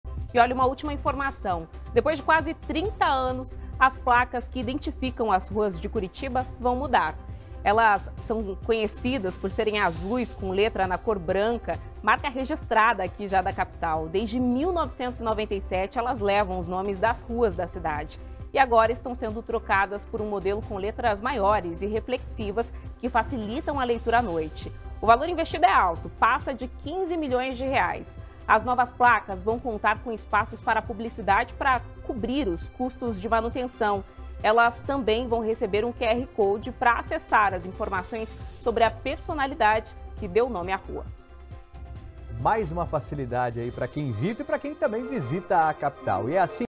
Placas de rua de Curitiba vão mudar
Depois de quase 30 anos as placas que identificam as ruas de Curitiba irão mudar